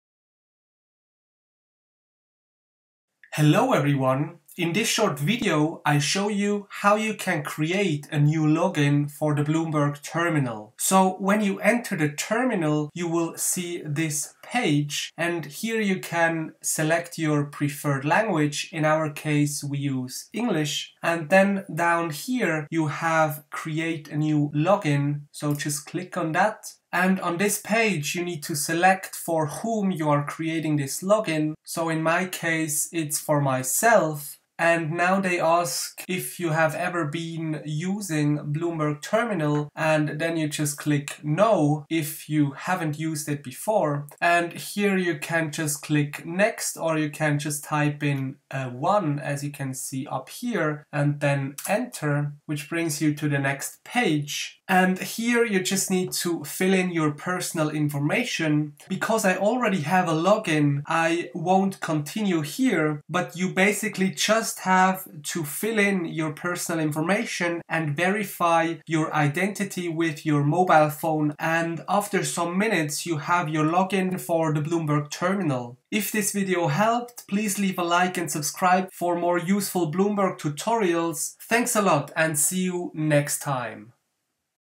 Create a new Bloomberg Terminal login
This short tutorial explains how to simply create a new Bloomberg Terminal login. If you need financial data for your university projects or thesis you mostly have access to Bloomberg at your University. To retrieve data, you need to first create your own login on the Terminal. This is done in few minutes. Just Open the terminal and click on create a new login. You need to verify your identity with your mobile phone.